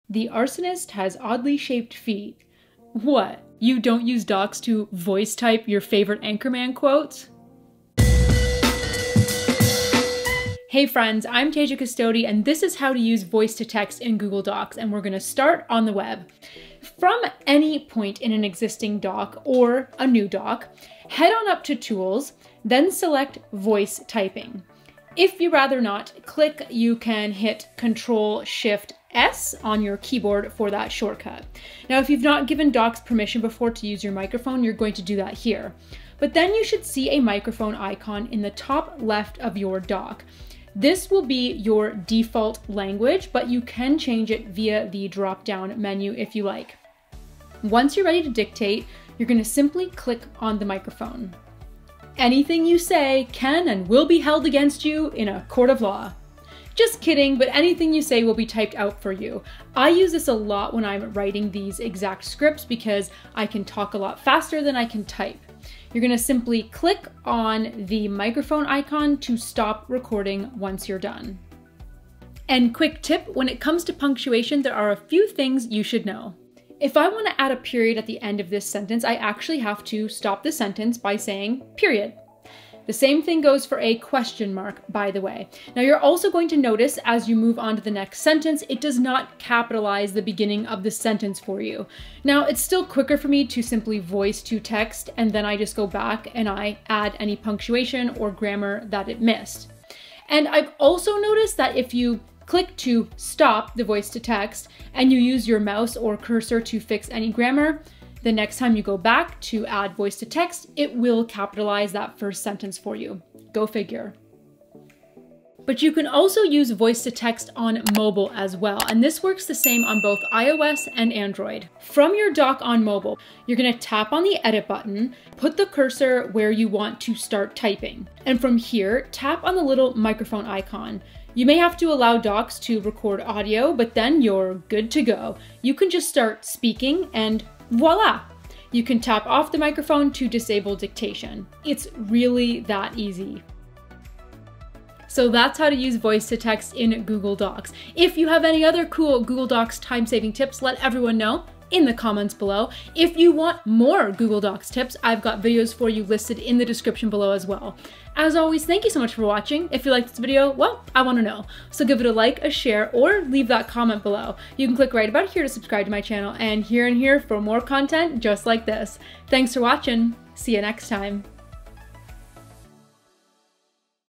How to Use Voice-to-Text in Google Docs
Hey friends! I’m Tasia Custode and in this video, I’m showing you how to use voice to text in Google Docs. This works both on the web and mobile. Plus, I’ll share a couple of punctuation tips.

⏰ TIMECODES:
00:00 - Voice to Text in Google Docs
01:55 - Docs Voice to Text Mobile

📷 YOUTUBE CAMERA GEAR:
Raleno LED Camera Light
🎙️Podcast Mic